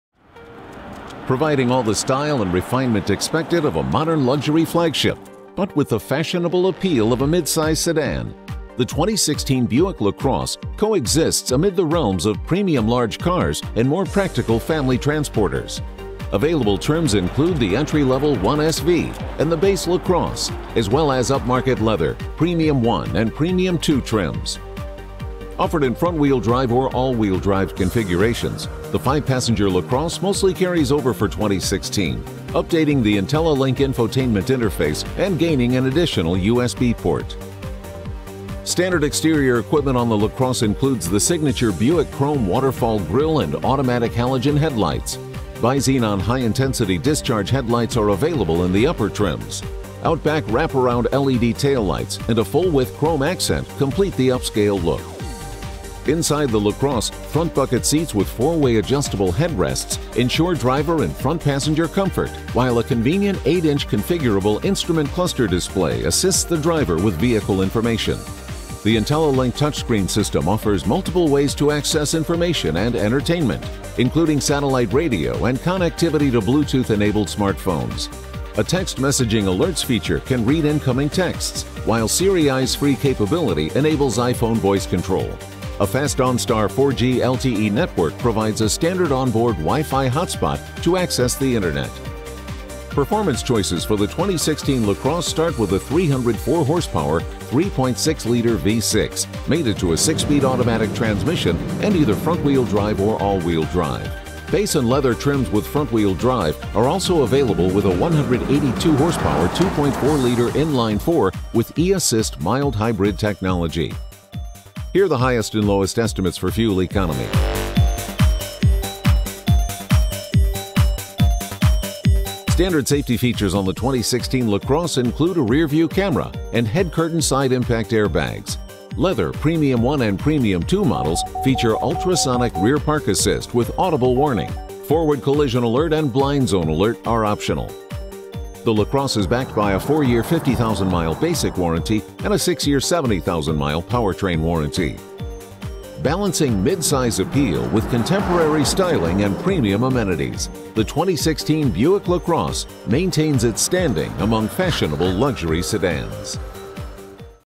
2016 Buick LaCrosse Test Drive
The 2016 Buick LaCrosse coexists amid the realms of premium large cars and more practical mid-size family transporters.